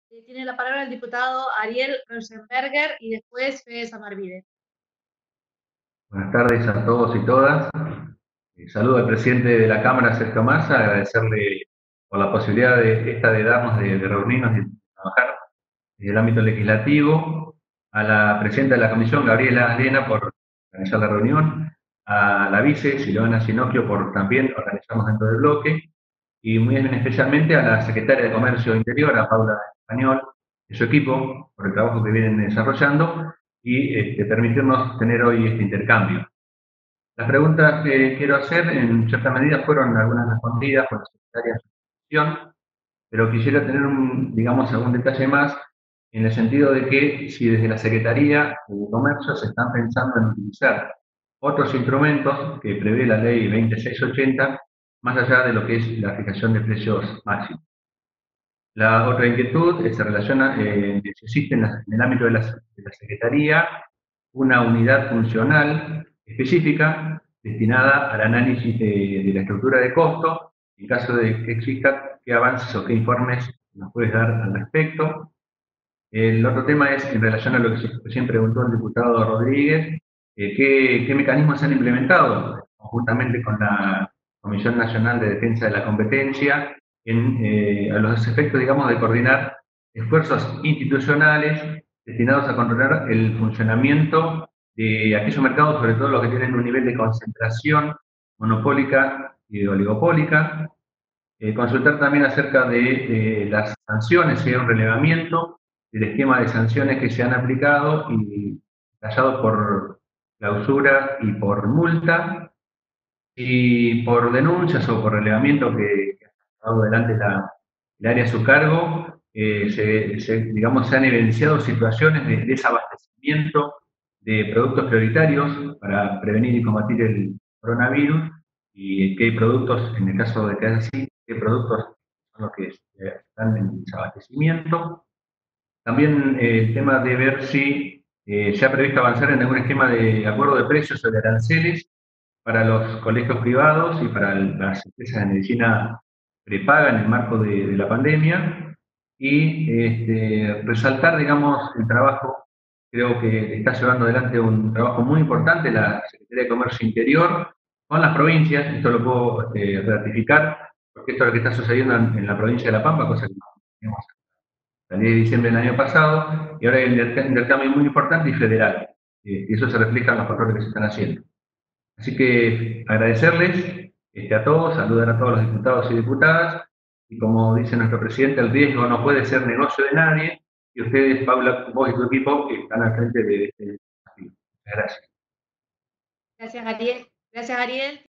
Diputado Rauschenberger, Ariel - Comisión de Comercio - 23-04-2020
Reunión Informativa por videoconferencia de la Comisión de Comercio con la participación de la Secretaria de Comercio Interior, Paula Español.